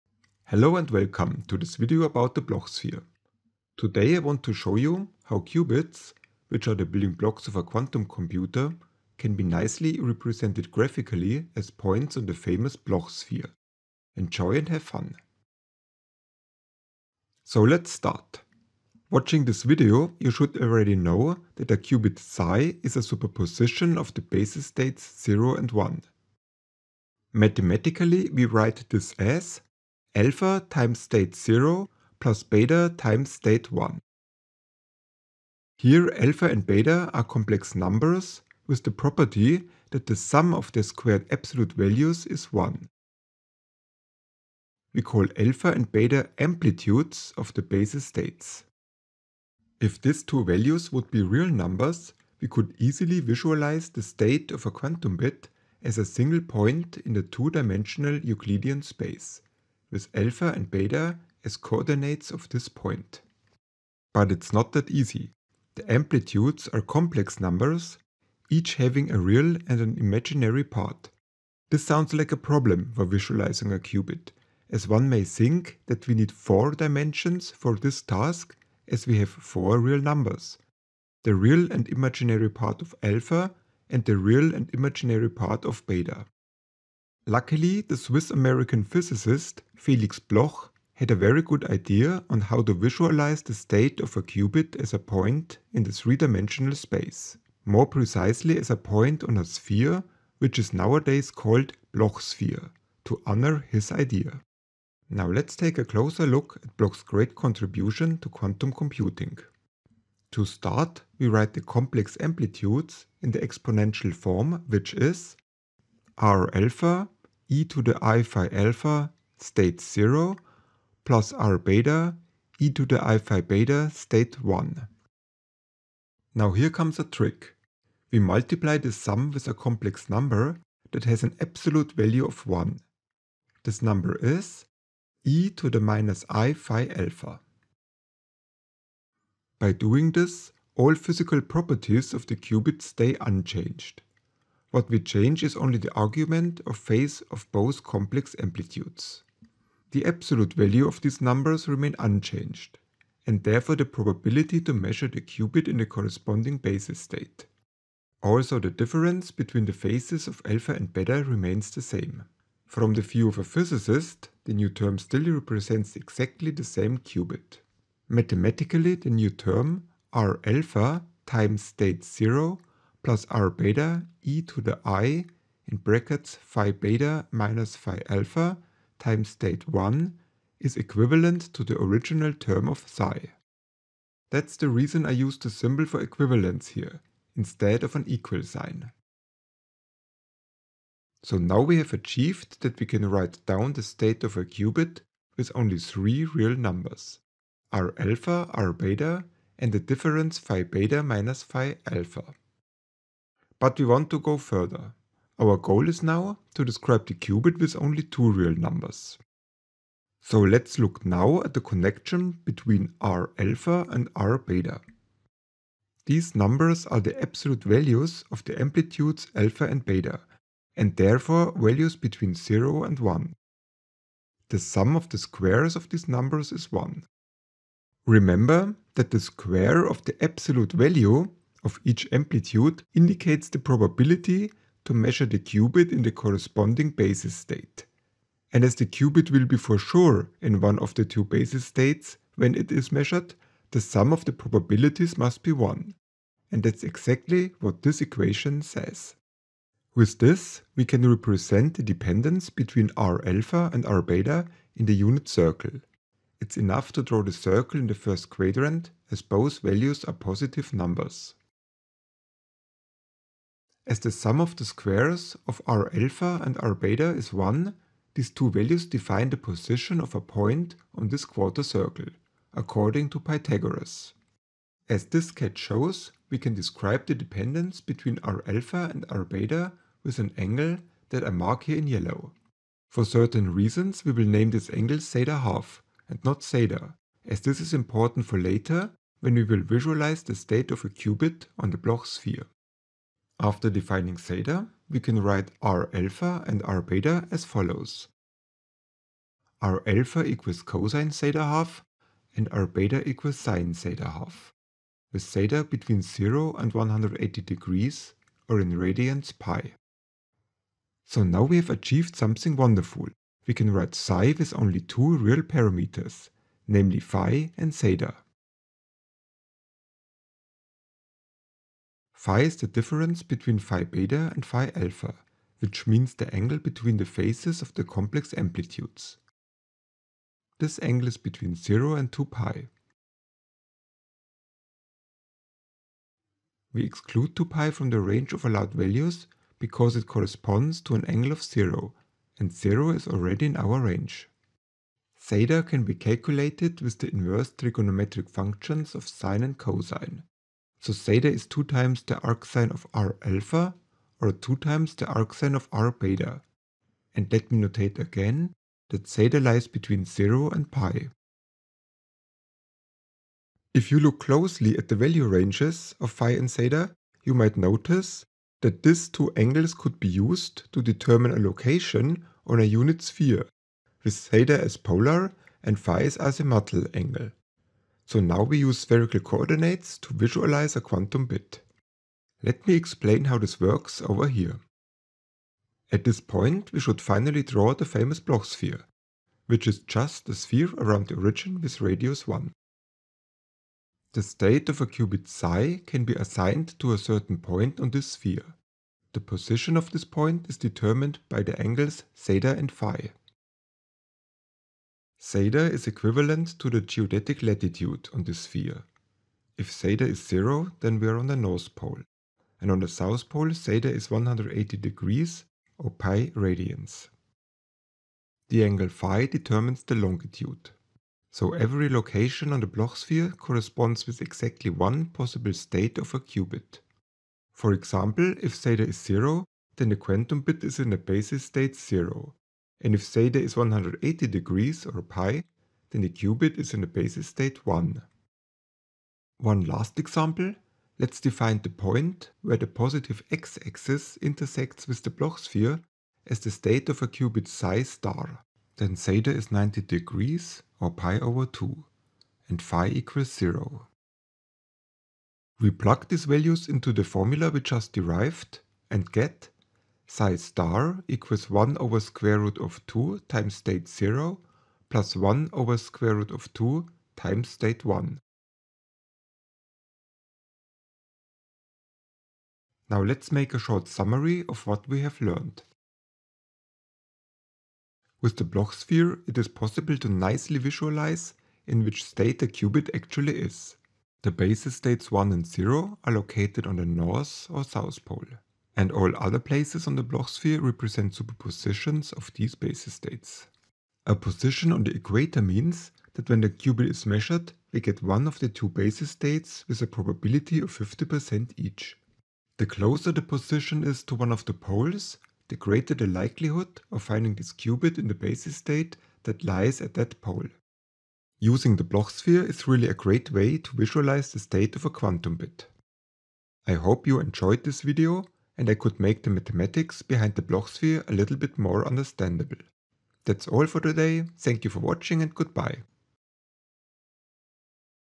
The Bloch Sphere (simply explained)
In this video I try to explain the math behind the famous Bloch sphere as simply and understandably as possible.